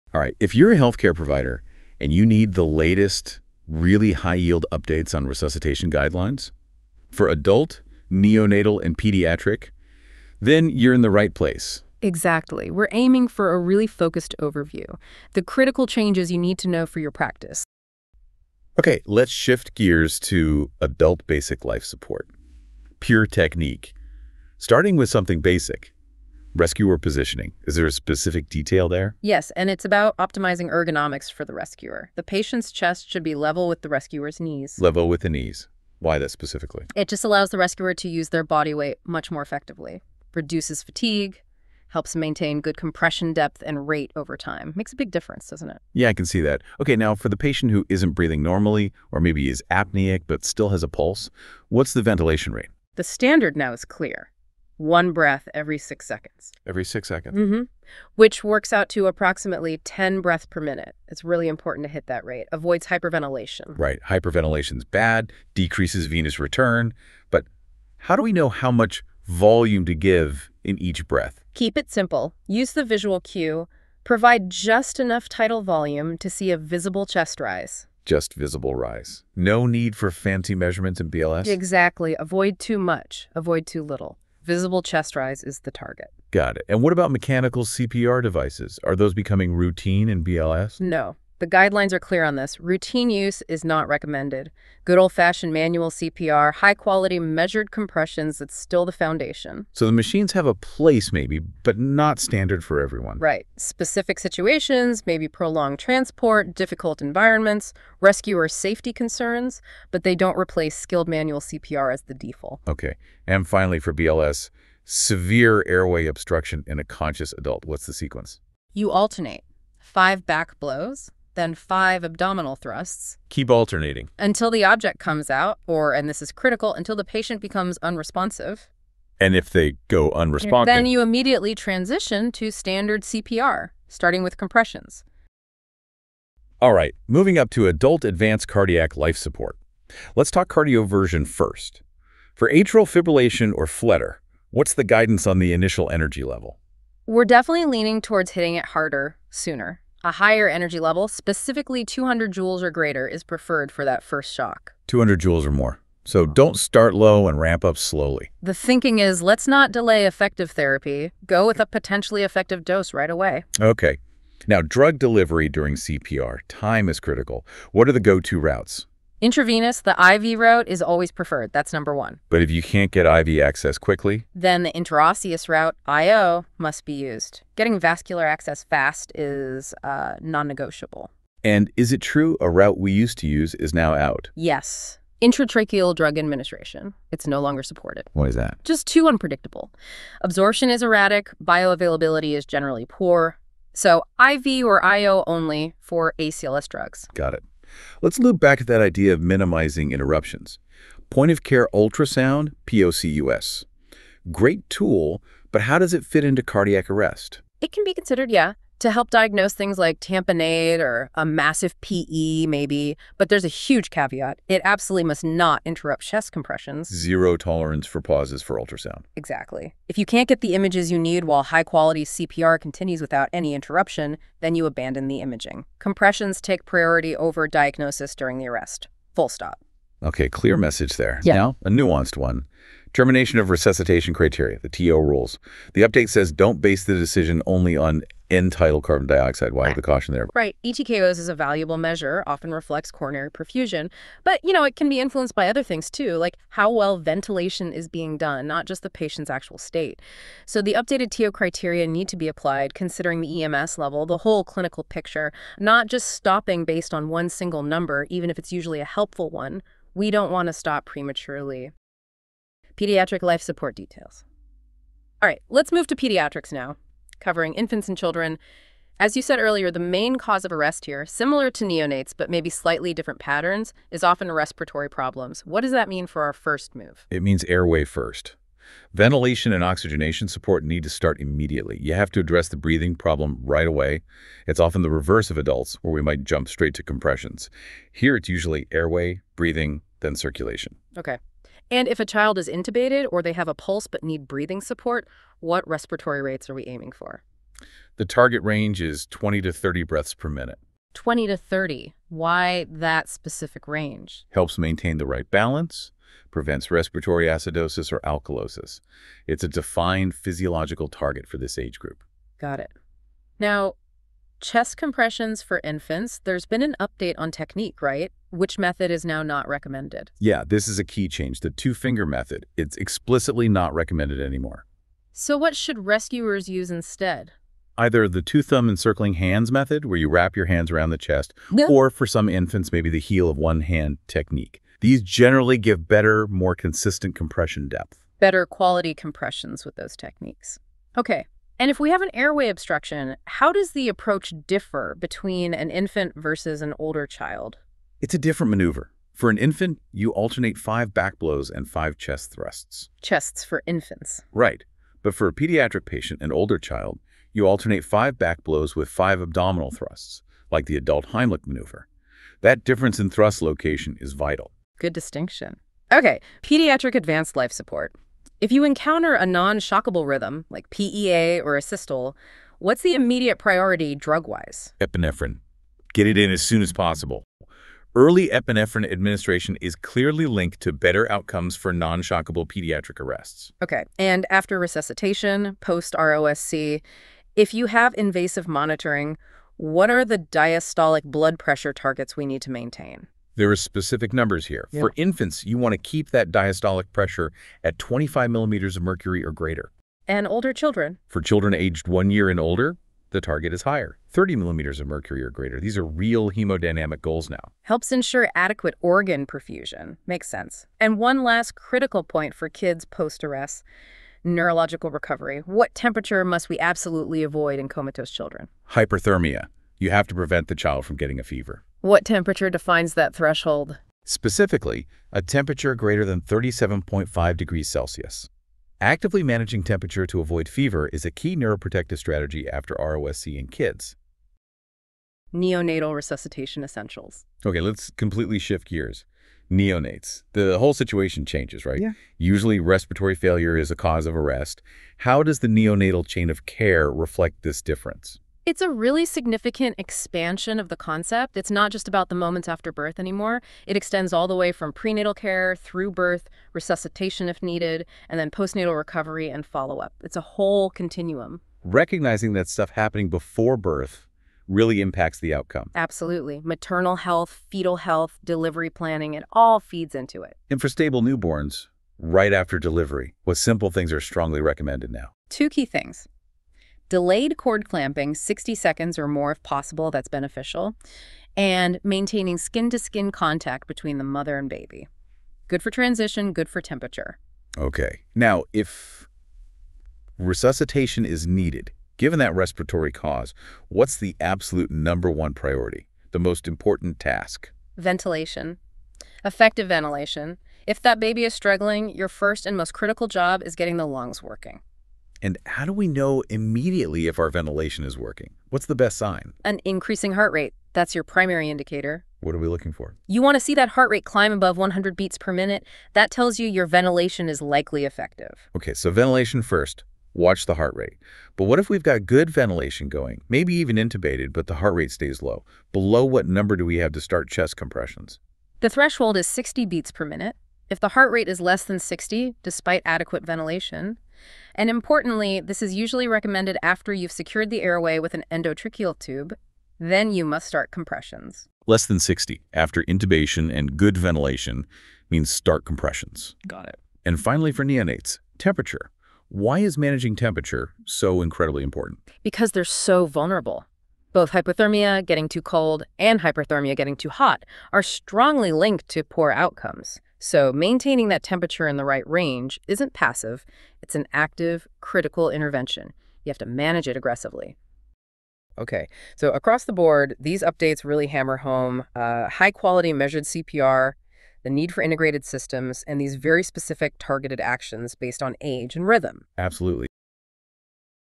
AHA 2025 update for adult, pediatric, and neonatal life support
Adult Basic Life Support (BLS)
- Initiate emergency care at the scene if high-quality CPR can be delivered safely.
- Single rescuer must activate the emergency response system before starting CPR.
- Position the patient’s chest level with the rescuer’s knees for effective compressions.
- Concurrent chest compressions and rescue breaths are recommended for cardiac/non-cardiac arrest.

- Provide enough tidal volume for visible chest rise; avoid hyperventilation and hypoventilation.
- Routine use of mechanical CPR devices is not recommended.
- If patient has a pulse but abnormal breathing: deliver one breath every six seconds (~10 breaths/min).
- Severe airway obstruction: alternate five back blows and five abdominal thrusts.

Adult Advanced Cardiac Life Support (ACLS)
- Rapid clinical stability assessment is essential, focusing on identifying signs of organ hypoperfusion.
- For atrial fibrillation or flutter cardioversion, 200 Joules or greater is preferred for the initial shock.
- Termination of Resuscitation (TOR) criteria are based on EMS level (BLS, ALS, Utstein-style).

- TOR decisions should not be based solely on ETCO₂ (end-tidal carbon dioxide) measurements.
- Effectiveness of double sequential defibrillation requires further study.
- Routine head-up CPR is not recommended outside of clinical trials.
- POCUS (Point-of-Care Ultrasound) may aid diagnosis but must not interrupt CPR.

Drug Administration and Rhythms
- Polymorphic ventricular tachycardia is an unstable rhythm requiring immediate defibrillation.
- The IV (intravenous) route is preferred for administering CPR drugs.
- The IO (intraosseous) route may be used if IV access cannot be established.
- Intratracheal drug administration is no longer supported in guidelines.
- Evaluating the underlying etiology helps guide the optimal ALS approach.

Neonatal Resuscitation
- The Neonatal Chain of Care extends from prenatal care through postnatal recovery and follow-up.
- Preparation is key: clinical teams must predetermine roles and continually train.
- For stable newborns, delayed cord clamping (≥ 60 seconds) and skin-to-skin contact are beneficial.
- Effective ventilation is the primary and most important task in neonatal resuscitation.
- An increasing heart rate is the best indicator of successful ventilation.

Neonatal Resuscitation
- If heart rate does not rise, troubleshoot ventilation and consider alternative airways (laryngeal mask or ET tube).
- Temperature management is critical; avoid both hypothermia and hyperthermia.
- Pulse oximetry guides oxygen administration and saturation targets.

Neonatal Resuscitation
- If heart rate remains less than 60 bpm despite adequate ventilation, start chest compressions immediately (recommended after successful intubation).
- If HR remains less than 60 bpm after effective compressions, administer epinephrine.
- IV route is preferred for epinephrine; temporary endotracheal administration is acceptable.
- If no heart rate after 20 minutes of interventions, discuss changes to care direction with the family.

Pediatric Basic Life Support (BLS)
- Respiratory problems are the leading cause of cardiac arrest in infants and young children.
- Prompt ventilation and oxygenation support must be started.
- Out-of-hospital arrest: combination of chest compressions and rescue breathing is recommended.

- Recommended respiratory rate: 20–30 breaths per minute for intubated patients or those with a pulse.
- Infant airway obstruction: alternate five back blows and five chest thrusts (abdominal thrusts not recommended for infants).
- Pediatric airway obstruction: alternate five back blows and five abdominal thrusts.

Pediatric CPR and Defibrillation
- If unresponsive with abnormal breathing: call for help and immediately start high-quality CPR.
- High-quality CPR defined by proper rate/depth, minimal interruptions, full chest recoil, and avoidance of hyperventilation.
- Infant chest compressions: use the two-thumb encircling or heel of hand method; two-finger method is not recommended.

- Early use of an Automated External Defibrillator (AED) is important.
- Pediatric pads/adapters are preferred when available.
- Minimize interruptions before and after timely defibrillation for ventricular fibrillation/pulseless VT.